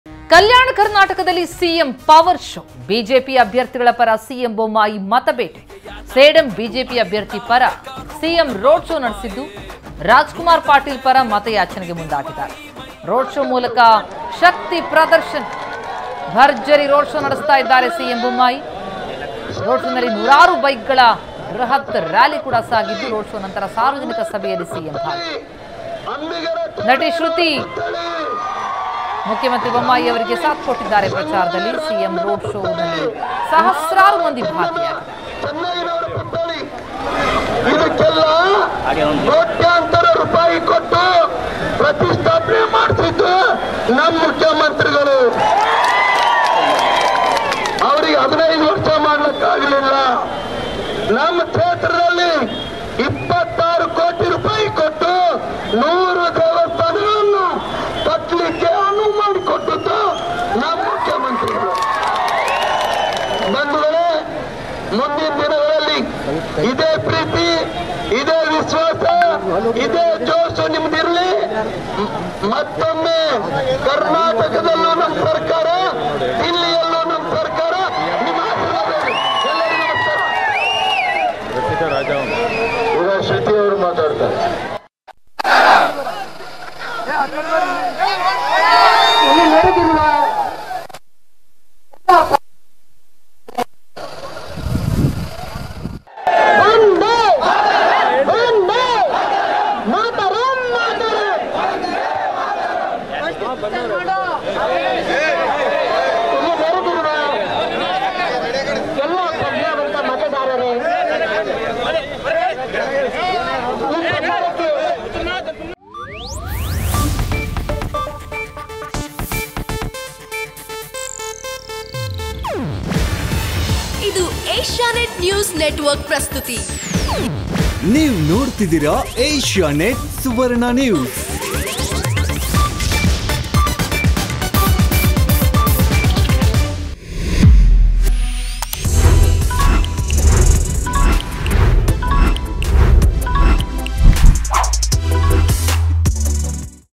CM Bommai And Actress Shruthi Hold Road Show For BJP Candidate Rajkumar Patil In Sedam
Suvarna News | ಸುವರ್ಣ ನ್ಯೂಸ್ | ಕನ್ನಡ ನ್ಯೂಸ್ | Suvarna News Today | Karnataka Political News |
Karnataka Political Updates 2023